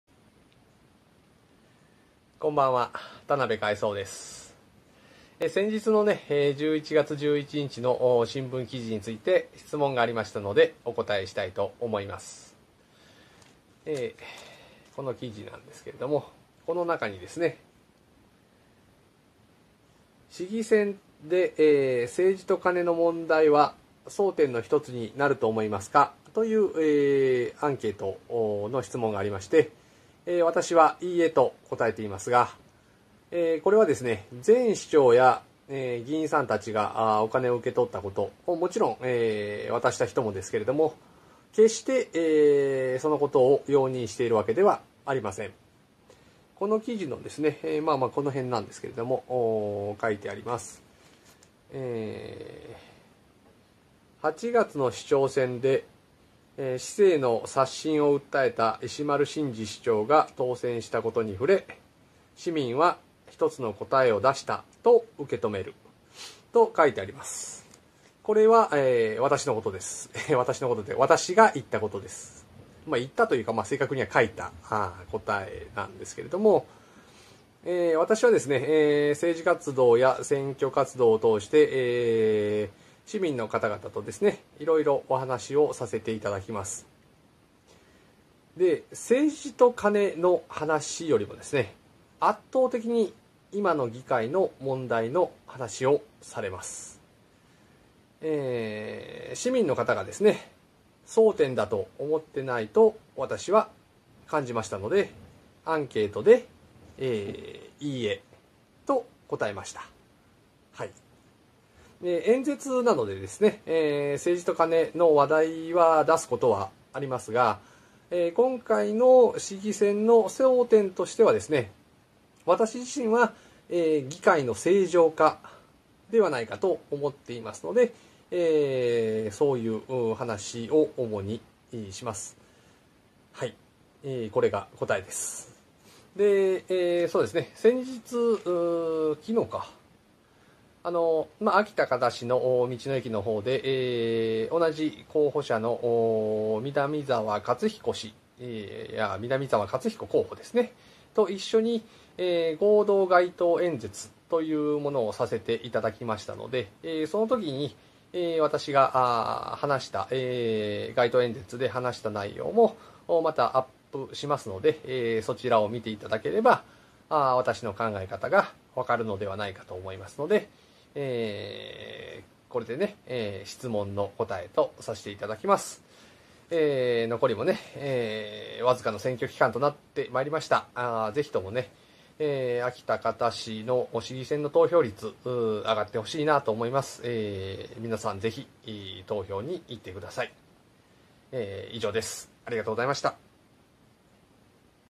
新聞記事についての質問にお答えしました
11/11日の新聞記事のアンケート内容について質問をいただきましたのでお答え致します